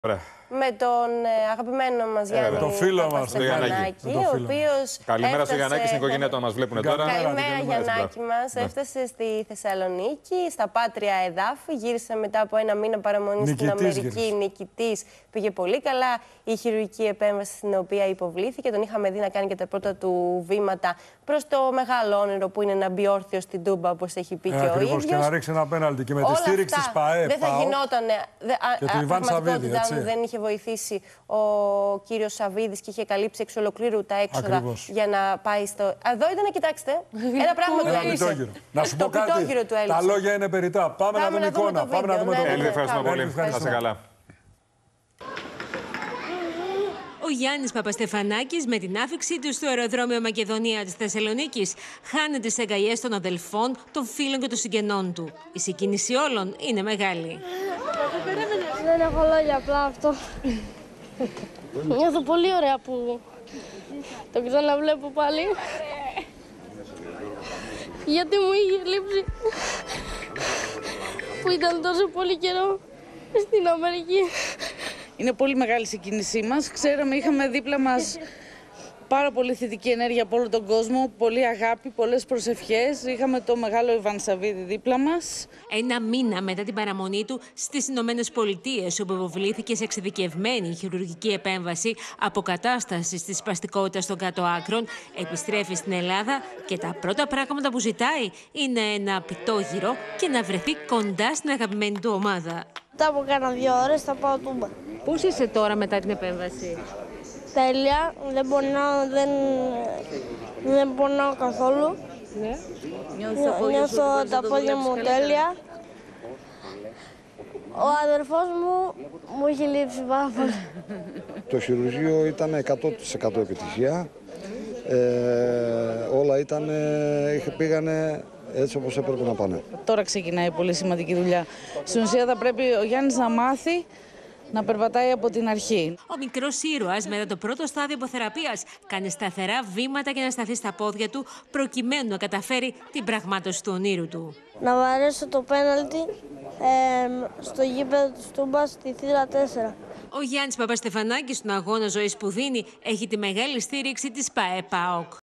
O μικρός Γιάννης Παπαστεφανάκης επέστρεψε νικητής στην Ελλάδα! | OPEN TV
Γίνετε μέλος στο κανάλι του OPEN για να αποκτήσετε πρόσβαση σε προνόμια:
Ο Γιαννακης επεστρεψε χθες στην Ελλάδα και συγκεκριμενα στη Θεσσαλονικη μετά τη λεπτή χειρουργική επέμβαση που υποβλήθηκε στην Αμερική. Σειρά τώρα έχει η σκληρή δουλειά για να σταθεί στα πόδια του και να κάνει το όνειρο του πραγματικότητα, που δεν είναι άλλο από το να μπει όρθιος στην Τούμπα.